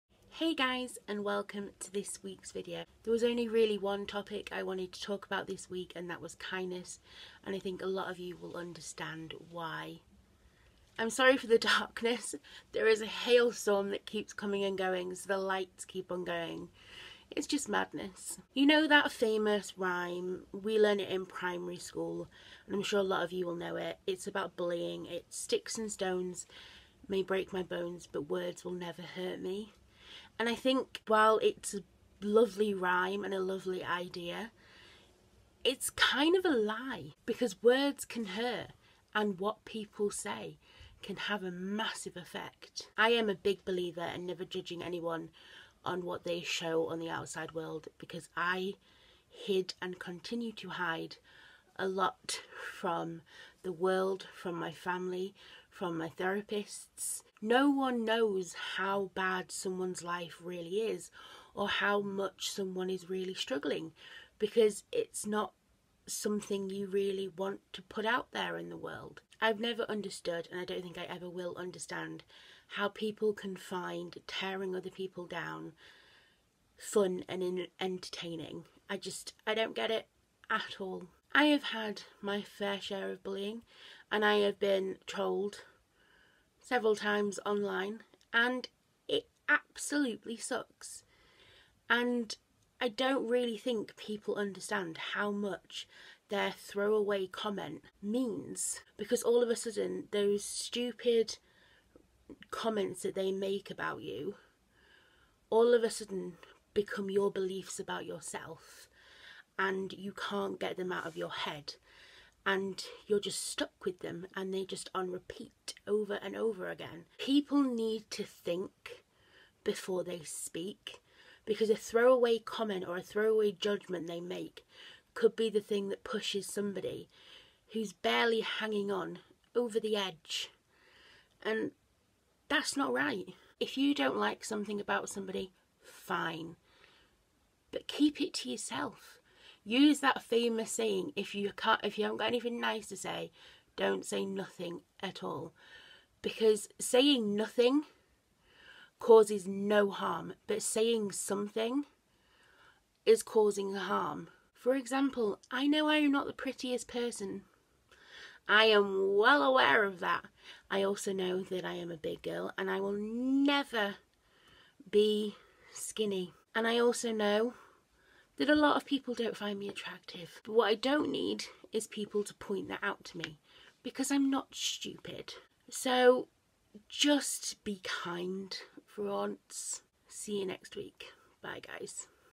Sprinkle of Courage - Be Kind!
Everyone is battling something emotional behind closed doors - that's life. In a world where you can be anything, be kind.

The Links|

Sprinkle of Courage

Personal